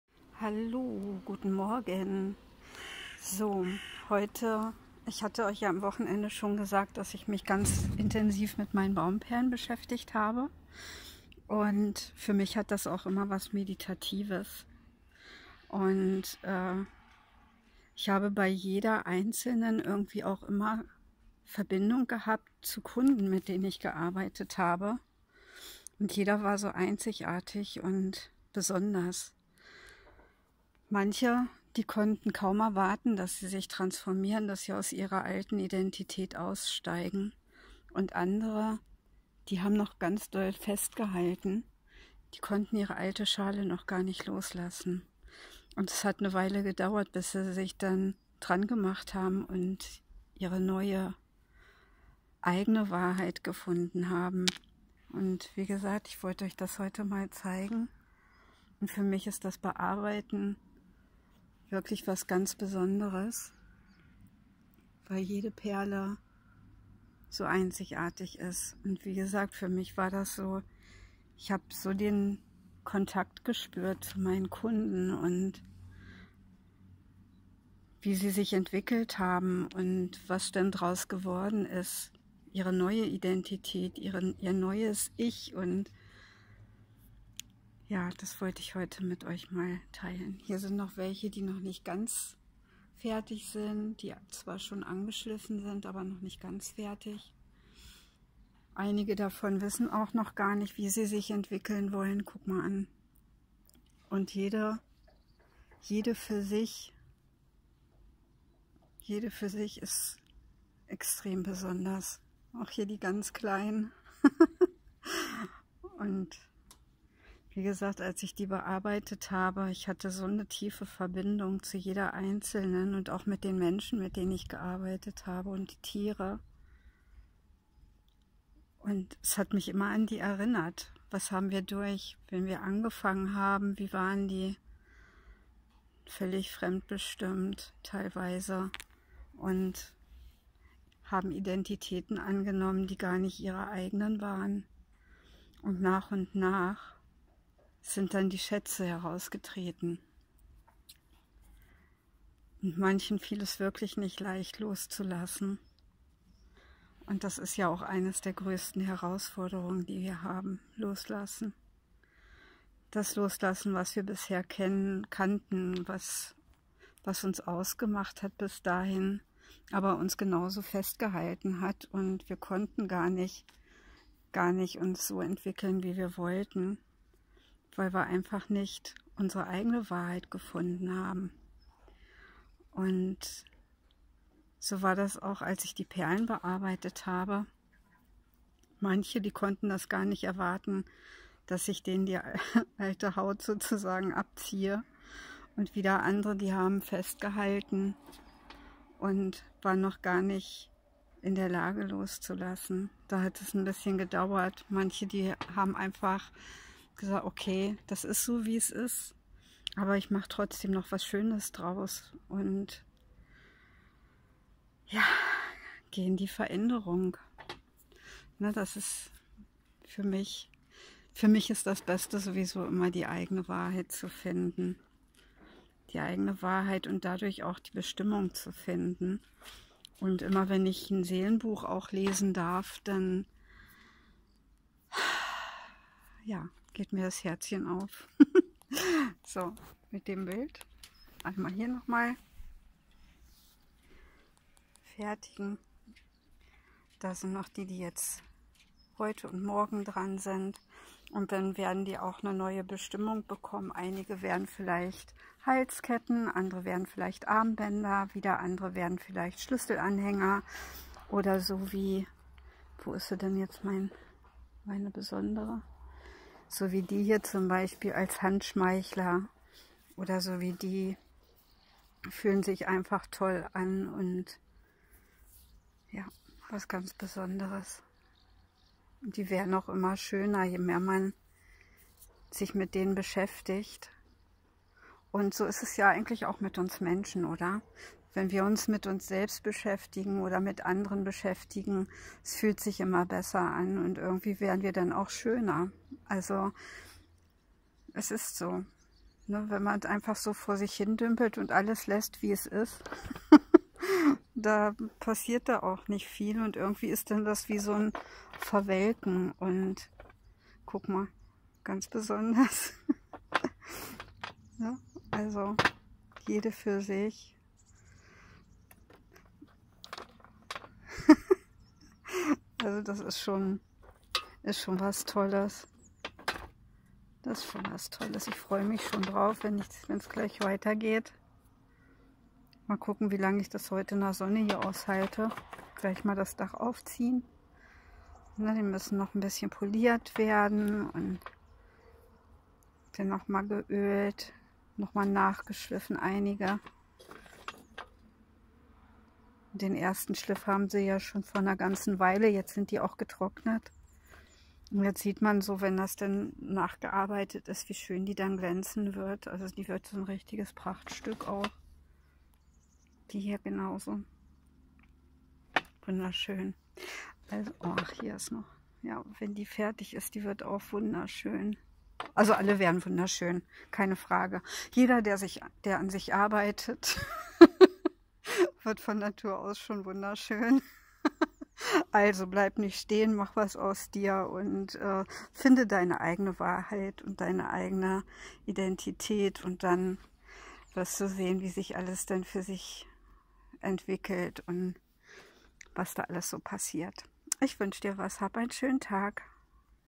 Baumperlen Video 2023 09 05 um 10 14 23
Beim Waldbaden kann man immer mal wieder Glück haben und Baumperlen finden.
Am vergangen Wochenende habe ich einige bearbeitet, was für mich auch immer ein meditativer Vorgang ist.
Das besondere diesmal war aber, dass ich mich mit meinen bisherigen Kunden bei der Bearbeitung verbunden gefühlt habe wie nie zuvor.
Ich weiß, wie schwer es manchen fällt ihre bisherige konditionierte Vergangenheit loszulassen.
Andere können es gar nicht erwarten ihre eigene Identität und Wahrheit zu finden.
Aber wie auch immer sie drauf waren, anschließend kamen sie sich selbst immer näher.
In diesem Sinne, wünsche ich dir einen zauberhaften Tag mit unendlichen Möglichkeiten, die mit dir selbst zu verbinden.
deine Carola